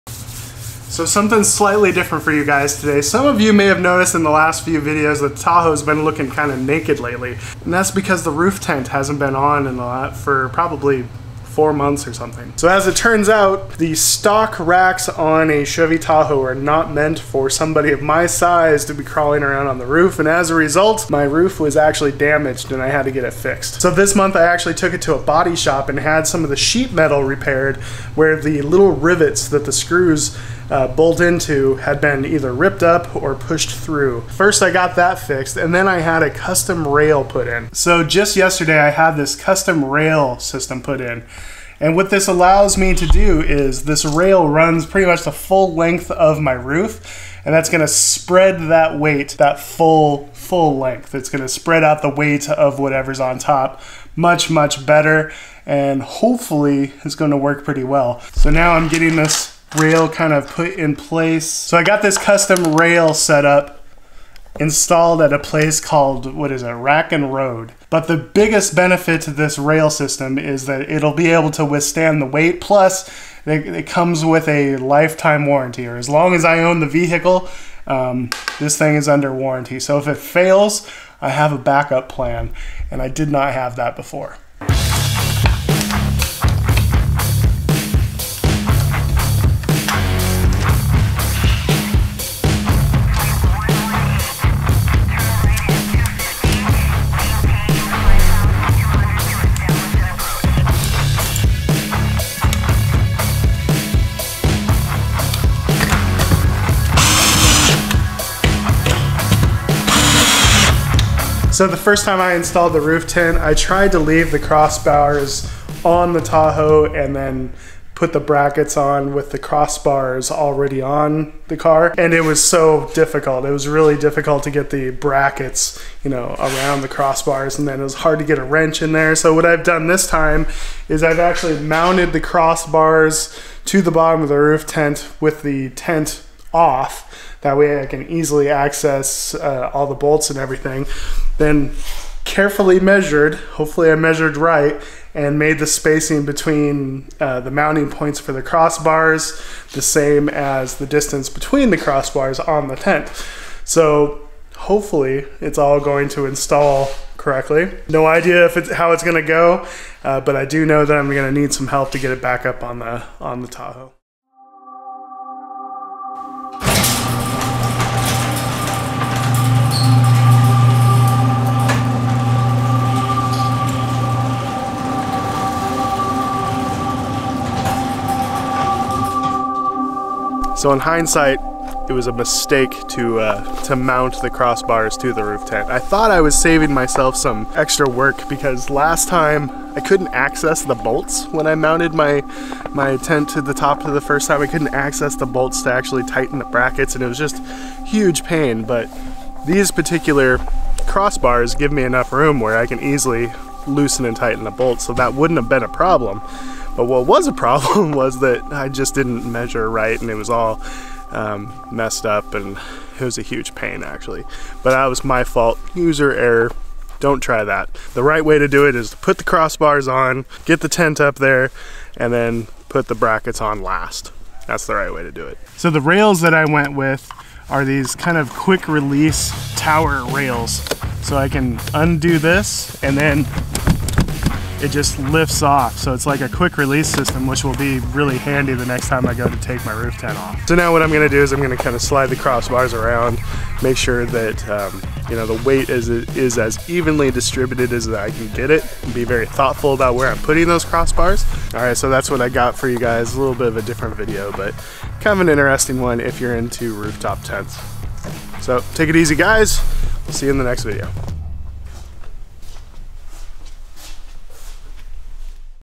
Roof tent update, What happened to the roof tent?
Some of you with a keen eye have noticed that my Roof tent hasnt been in on top of my tahoe for quite a while... here is why, and here is the fix.

I had a custom rail system installed at Rack and Road in Bellvue washington.